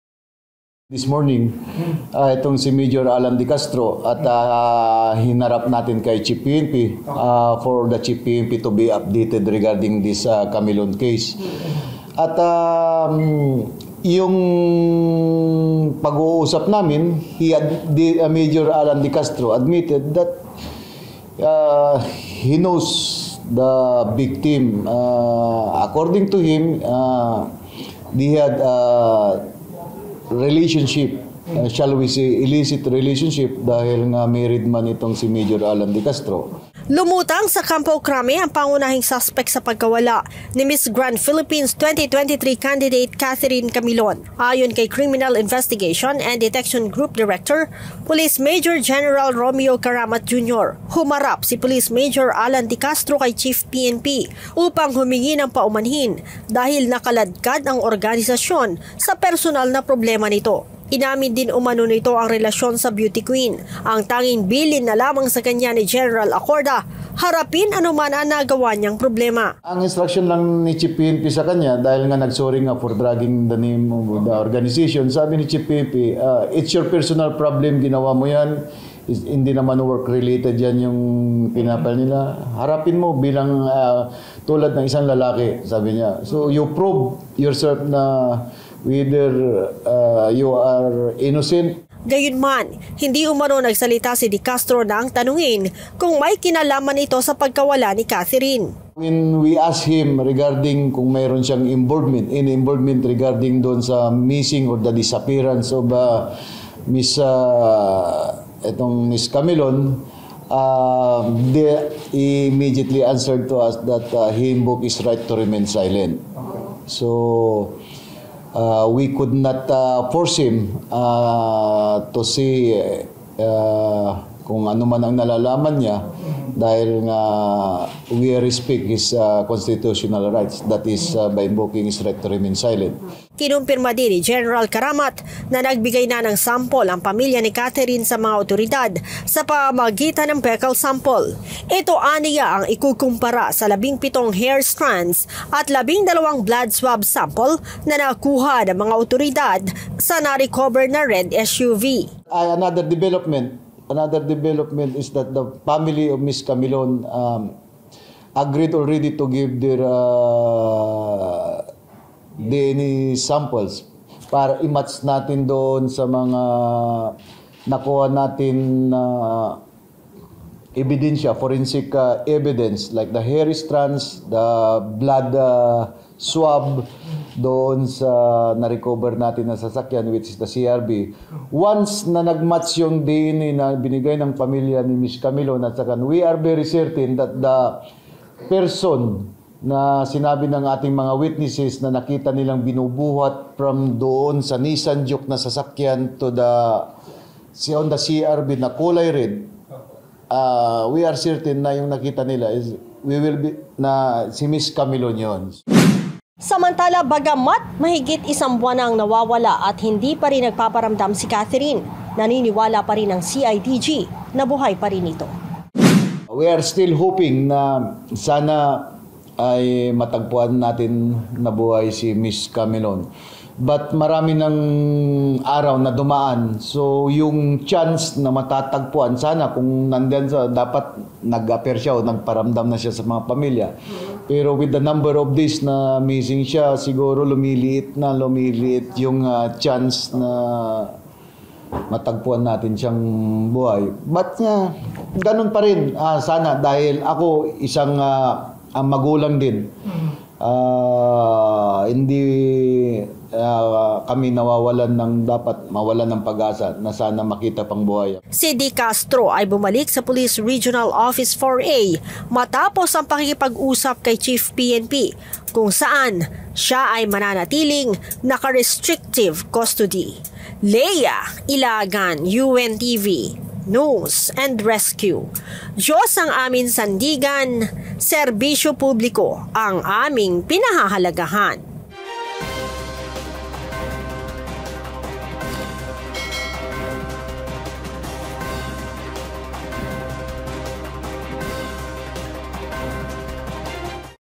Pulis na dawit sa pagkawala ng beauty queen, inamin ang relasyon; humingi ng paumanhin kay PNP chief
Lumutang sa Kampo Crame at humingi ng paumanhin kay Philippine National Police chief Gen. Benjamin Acorda ang pulis na dawit sa pagkawala ng beauty queen na si Catherine Camilon.

Ayon sa Criminal Investigation and Detection Group, inamin din ng nasabing pulis ang  relasyon kay Camilon pero tikom ang bibig nito nang tanungin kung may kinalaman siya sa pagkawala ng dalaga.